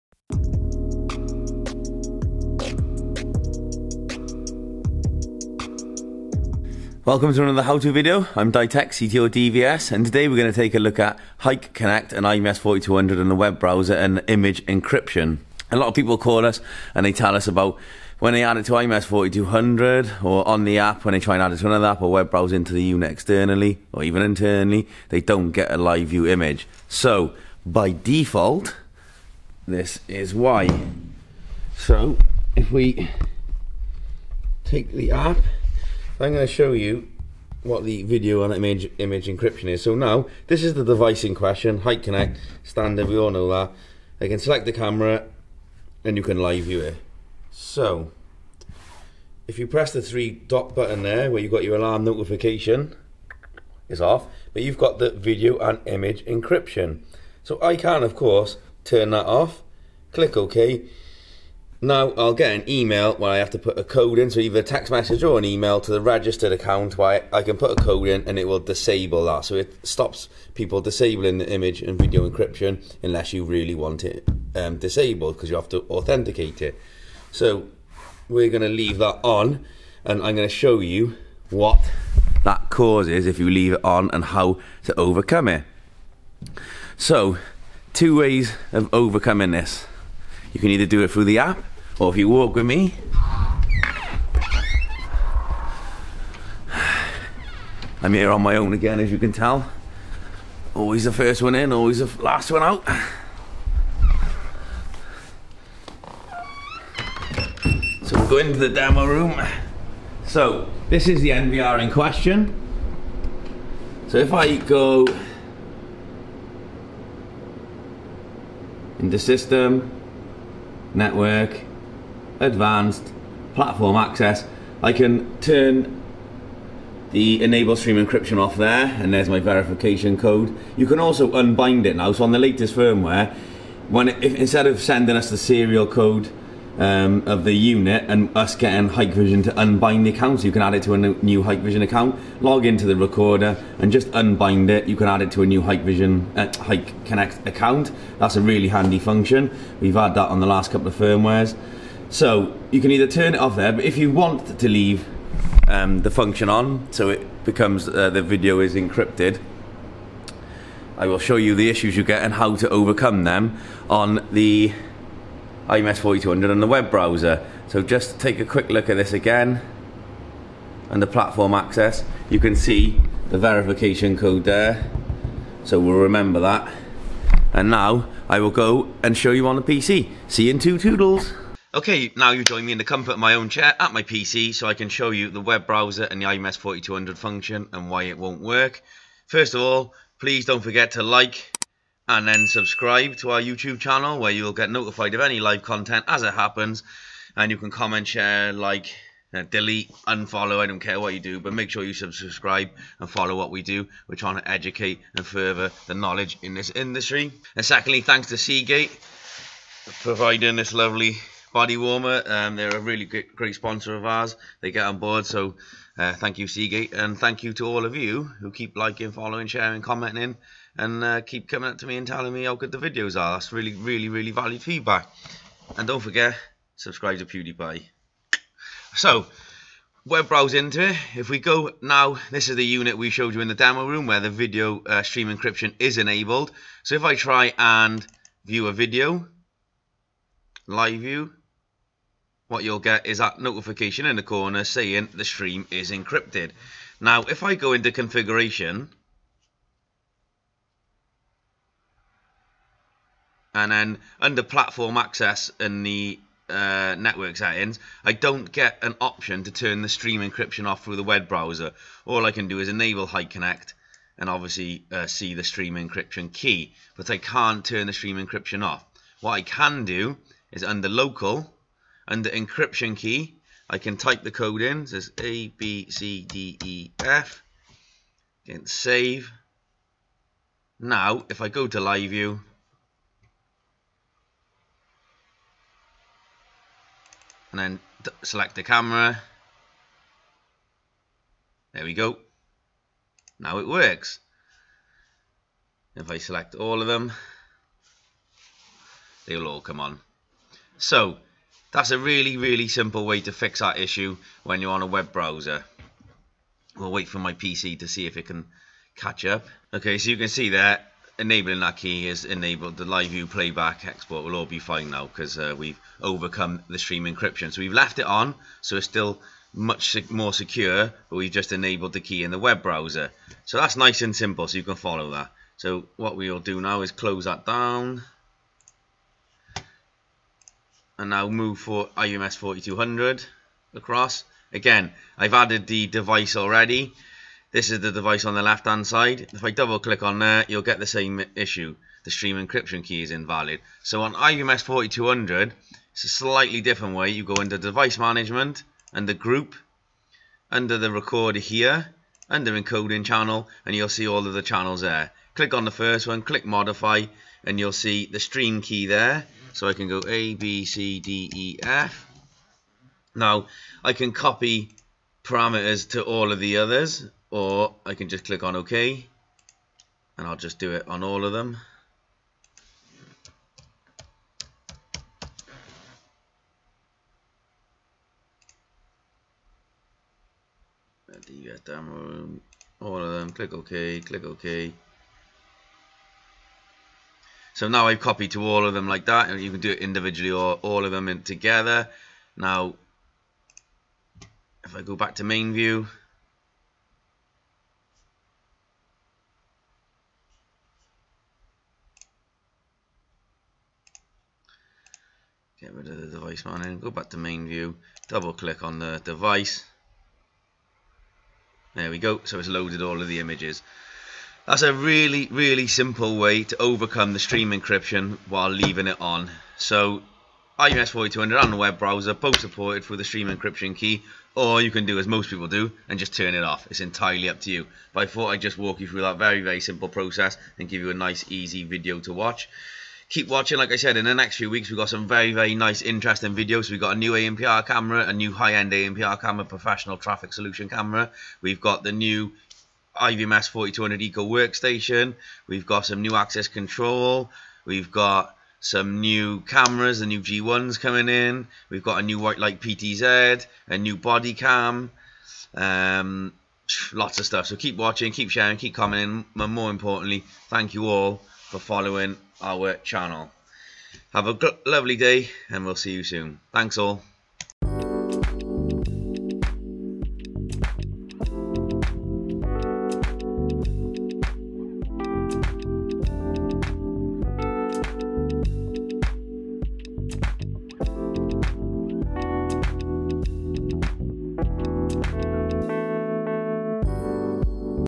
Hikvision Hik-Connect Stream Encryption Key
In this video we take a look at the Hikvision HIK-Connect Stream Encryption Key Function.

We show you how to turn this function off or keep it enabled. We can add the encryption key to a web browser or IVMS-4200 to get live view and playback.

I am sure we have all been caught out by this! We get asked this all the time so hopefully this will really help you out!

Powered by Seagate Skyhawk